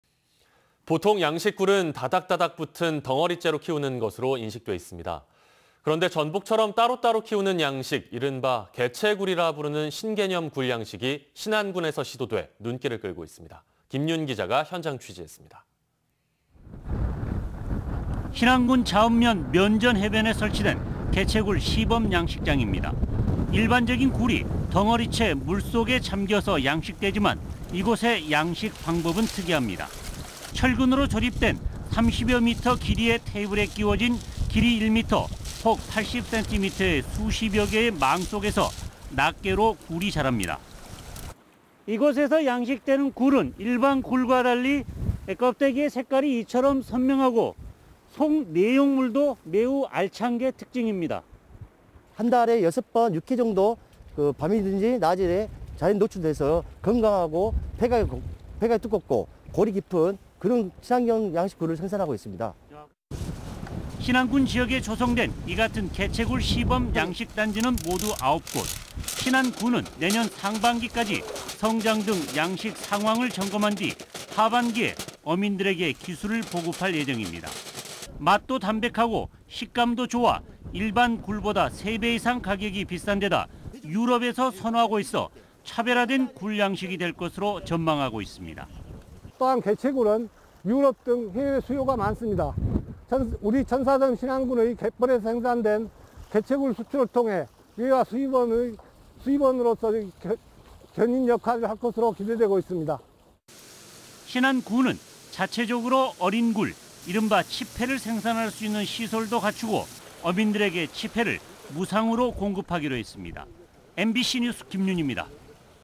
'전복처럼 따로 따로'.. 신개념 굴양식 도전-R (210203수/뉴스데스크)
◀ＡＮＣ▶

 양식굴이라고 하면 다닥다닥 붙은 덩어리째
키우는 것으로 인식돼 있습니다.

 그런데 전복처럼 따로 따로 키우는 양식,
이른바 '개체굴'이라 부르는 신개념 굴 양식이
신안군에서 시도돼 눈길을 끌고 있습니다.

 김 윤 기자가 현장 취재했습니다.
           ◀ＥＮＤ▶

 신안군 자은면 면전해변에 설치된 개체굴
시범 양식장입니다.

 일반적인 굴이 덩어리 채 물 속에 잠겨서
양식되지만 이곳의 양식 방법은 특이합니다.

 철근으로 조립된 30여 미터 길이의
테이블에 끼워진 길이 1미터 폭 80센티미터의 수십여 개의 망 속에서 낱개로 굴이 자랍니다.

(기자출연) 이곳에서 양식되는 굴은
일반 굴과 달리 껍데기의 색깔이 이처럼
선명하고 내용물도 매우 알찬게 특징입니다.

◀ＩＮＴ▶신상수 신안군 수산담당
"한 달에 6번씩 6-7일 정도 밤이나 낮에
자연노출돼서 건강하고 패각이 두껍고 골이
깊은 그런 친환경 양식 굴을 생산하고 있습니다."

 신안군 지역에 조성된 이같은 개체굴 시범양식단지는 모두 9곳.

 신안군은 내년 상반기까지 성장 등 양식상황을 점검한 뒤 하반기에 어민들에게 기술을 보급할 예정입니다.

 맛도 담백하고 식감도 좋아 일반굴보다
3배 이상 가격이 비싼데다 유럽에서 선호하고 있어 차별화된 굴양식이 될 것으로 전망하고
있습니다.

◀ＩＮＴ▶박영철 신안군 산업건설국장
"개체굴은 유럽 등 해외수요가 많습니다. 우리 천사섬 신안군의 갯벌에서 생산된 개체굴 수출을 통해 외화수입원의 견인 역할을 할 것으로 기대하고 있습니다."

 신안군은
자체적으로 어린 굴, 이른바 치패를 생산할 수 있는 시설도 갖추고 어민들에게 치패를
무상으로 공급하기로 했습니다.

MBC NEWS 김윤입니다. ///